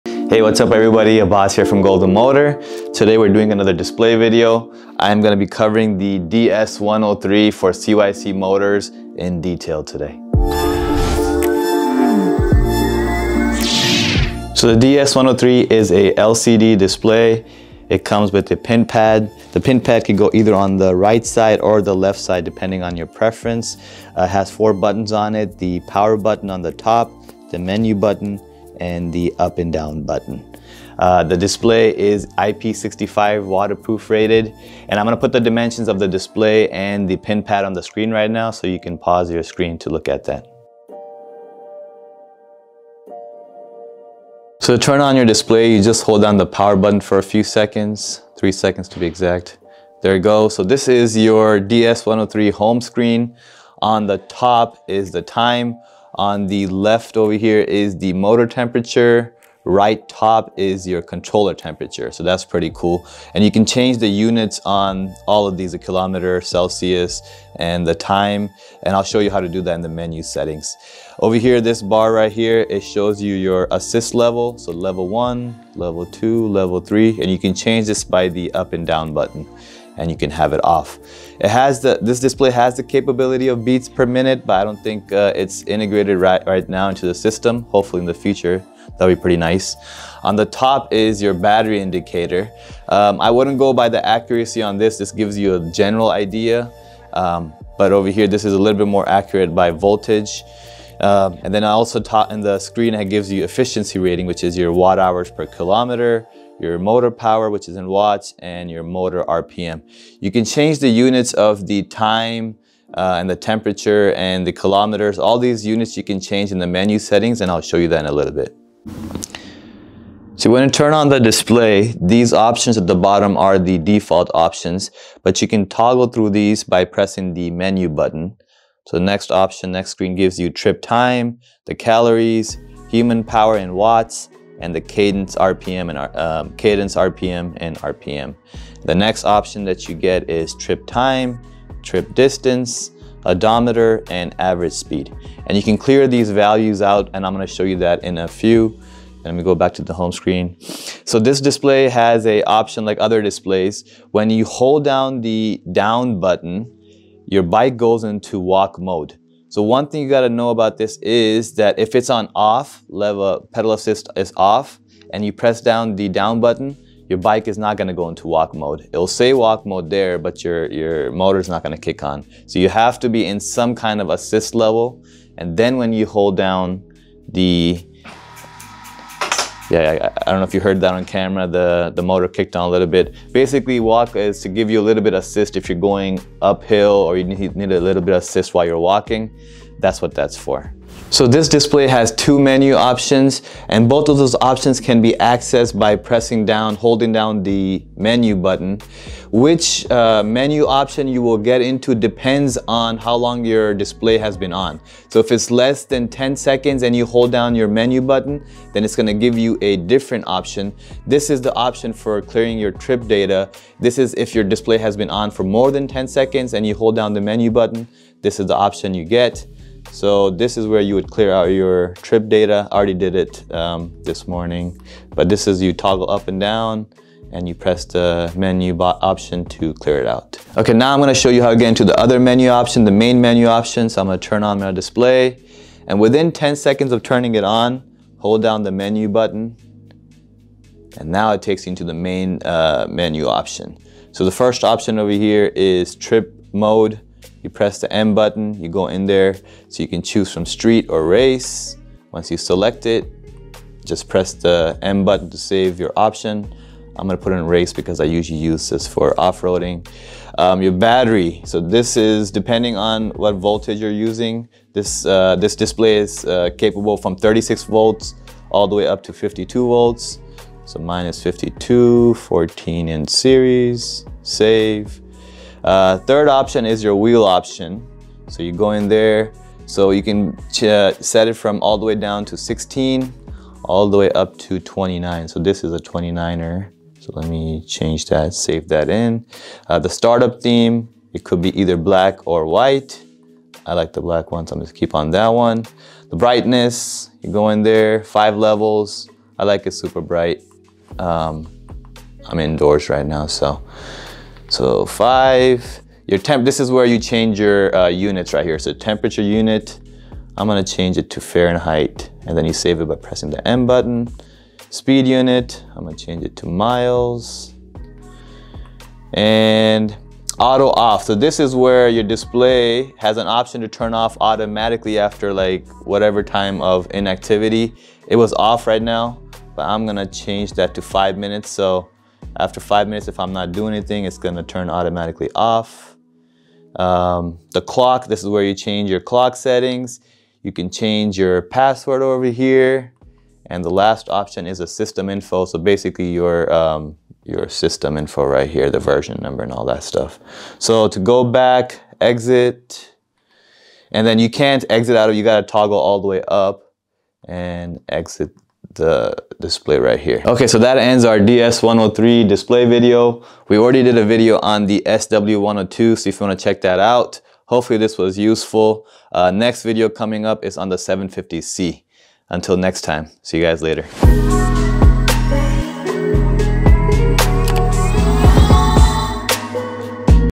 CYC DS103 Display
Detailed tutorial on the DS103 Display for the CYC Motors.